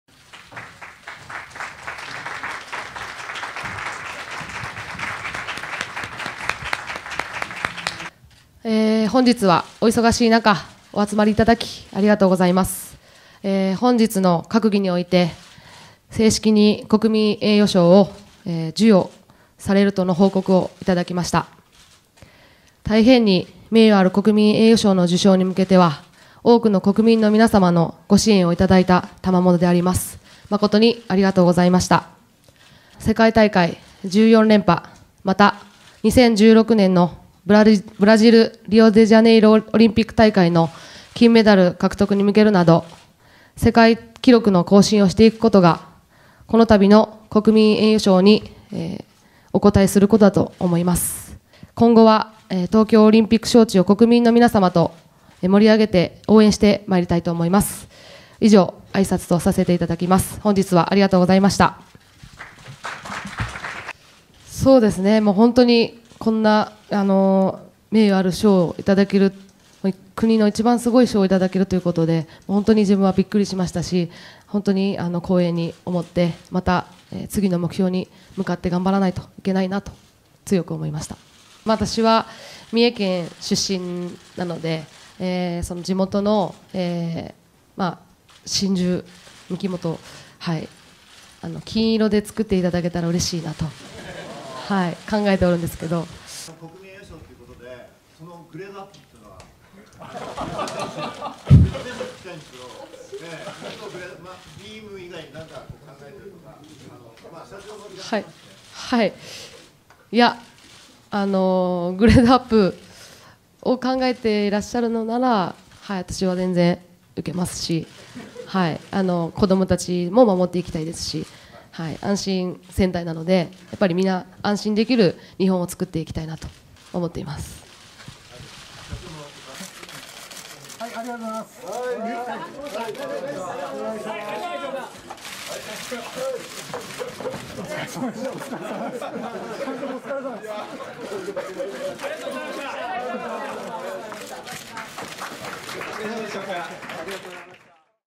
吉田沙保里選手に国民栄誉賞決定 「記録更新で賞に応える」
野田佳彦首相は１０月２３日、レスリング女子で、五輪と世界選手権を合わせて１３連覇を達成した吉田沙保里選手に対し、国民栄誉賞を授与することを正式に決めた。１１月７日に首相官邸で表彰式を行う。